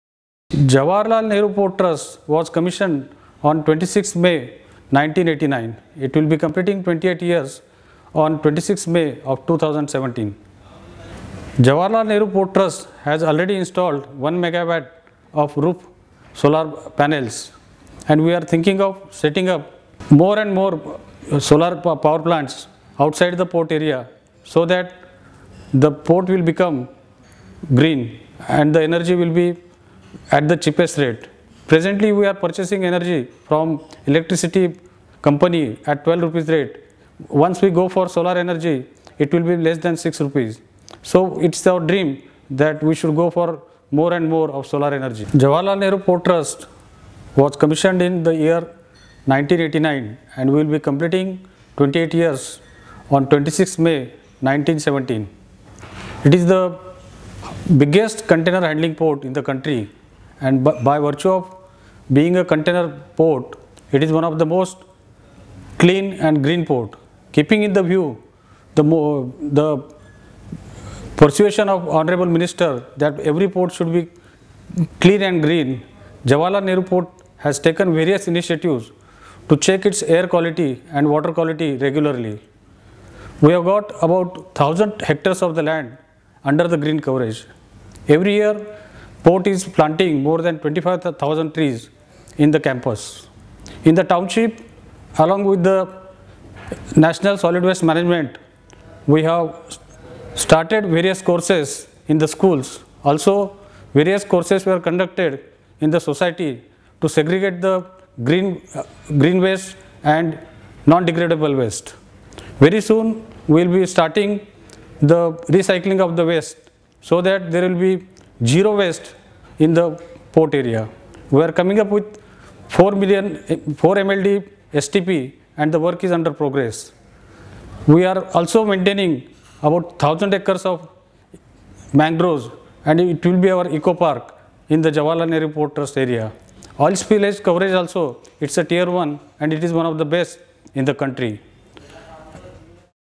JNPT's vision on Green Port & Oil Spill Management Conclave 2017
Shri Anil Diggikar, Hon'ble Chairman JNPT shares his vision on JNPT's committment to Green Port philosophy.